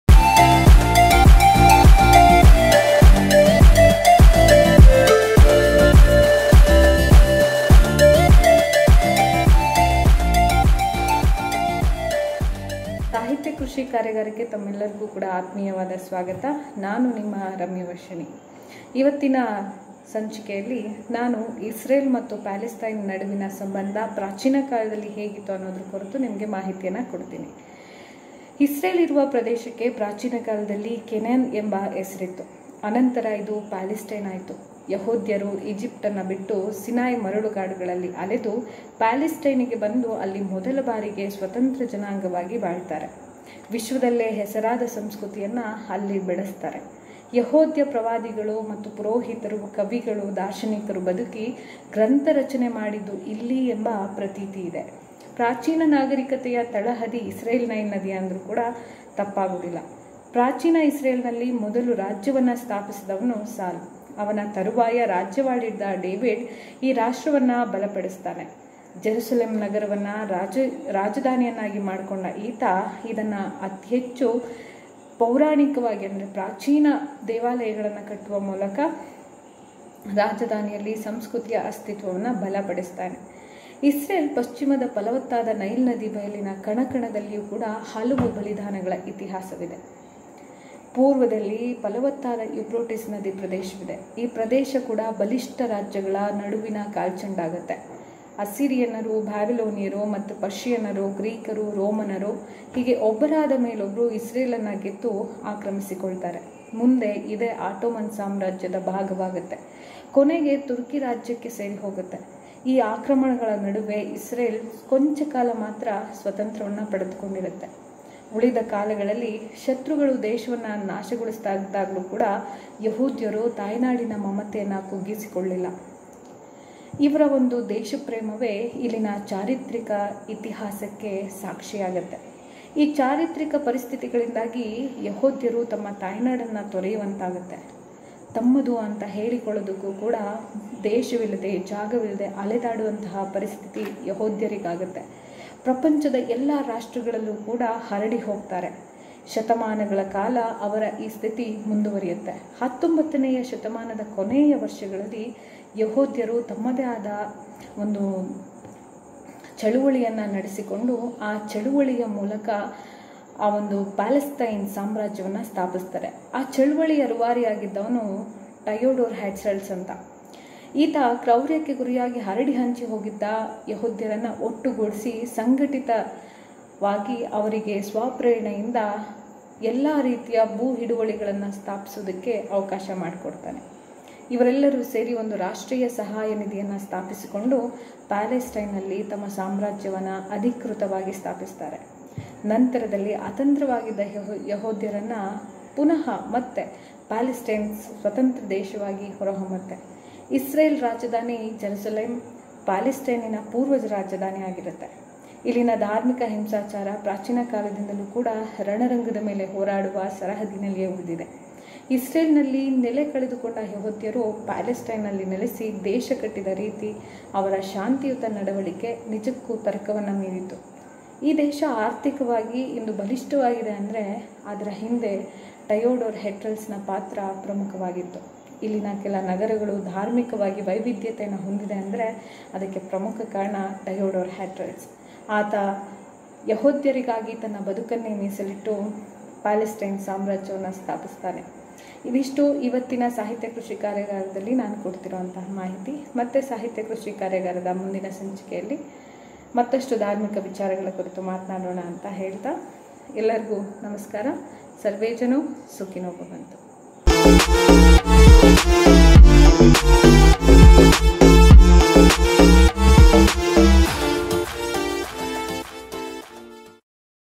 ಪ್ಯಾಲೆಸ್ಟೈನ್ -ಇಸ್ರೇಲ್ ಪ್ರಾಚೀನ ಭಾಷೆ ಸಂಸ್ಕೃತ ಆಗಿತ್ತಾ?ಯಹೋದ್ಯರ ಸ್ಥಿತಿ ಹೇಗಿತ್ತು?ಸಾಹಿತ್ಯ ಕೃಷಿ ರಮ್ಯವರ್ಷಿಣಿ
ಆ ನಾಯಕನಿಂದ ನೆಲೆ ಕಂಡುಕೊಂಡ ಯಹೋದ್ಯರು ? ಪ್ಯಾಲೆಸ್ಟೈನ್ ರಚನೆಯ ಕಾರಣವೇನು? ಚಳುವಳಿಯ ಹರಿಕಾರನಾದ ಟಯಡೋರ್ ಹೆಟ್ರಲ್ಸ್